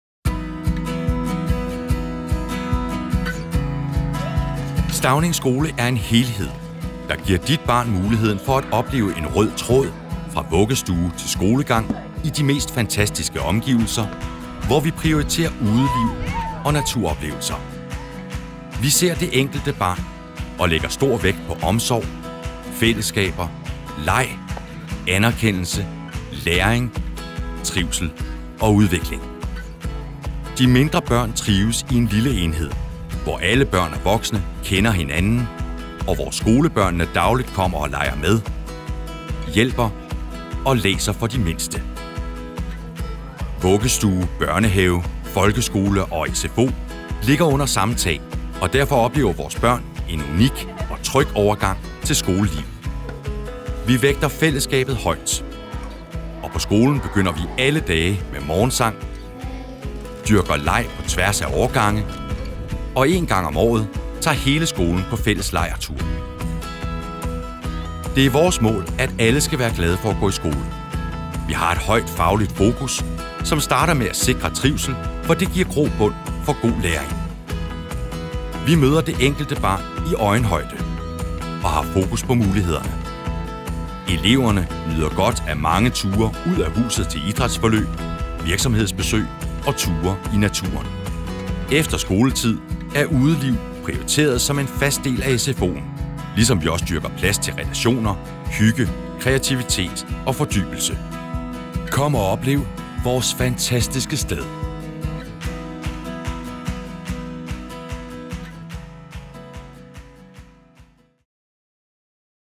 Stauning Skole
Film om Stauning Skole, SFO, Børnehave og Vuggestue